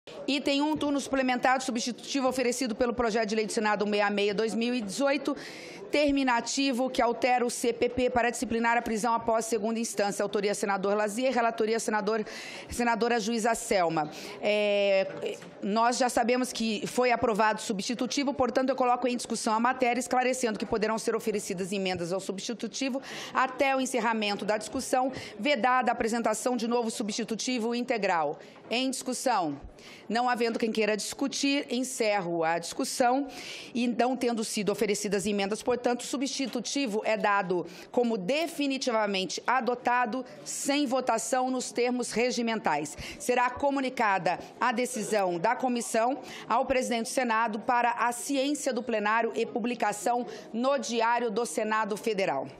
Em turno suplementar, CCJ aprova prisão em segunda instância
A Comissão de Constituição e Justiça aprovou, em turno suplementar, o projeto ( PL 166/2019) que disciplina a prisão após a condenação em segunda instância. Por ser um substitutivo, a matéria precisava passar por mais uma votação na CCJ.